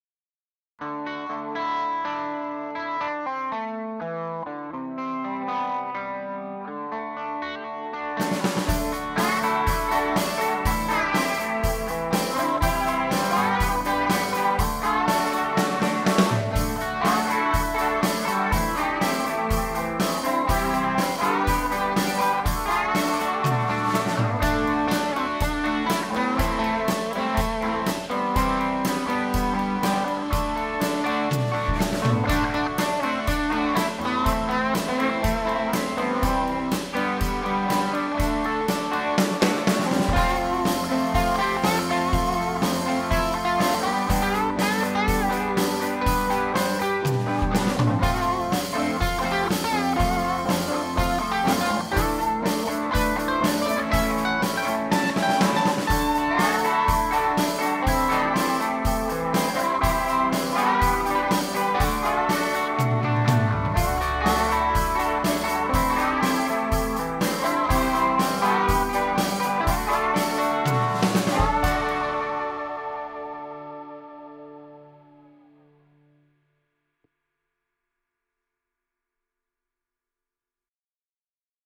Hammond Leslie G Pedal
Short but sweet video of the new compact version from Hammond.